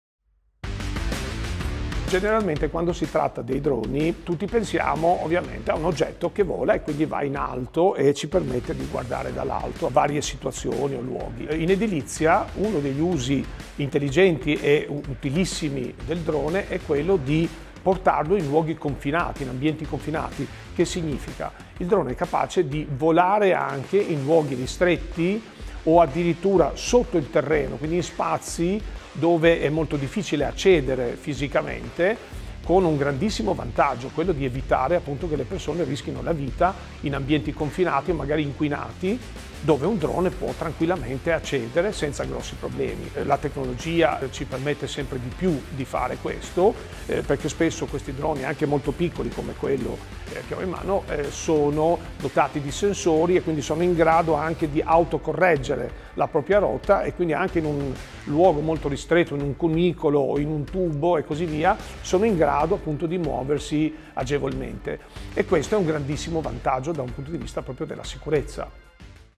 Il drone negli ambienti confinati
Protagonista della puntata odierna della nostra rubrica EDILI.COM  è... il DRONE!

Utilissimo per sorvolare e indagare luoghi d'interesse tecnico e cantieri, il drone è anche un grande alleato per la SICUREZZA negli ambienti di lavoro che presentano rischi estremi, per esempio gli spazi confinati.

Ascoltiamo il nostro architetto Gazmend Llanaj nella pillola informativa su questo specifico utilizzo del drone!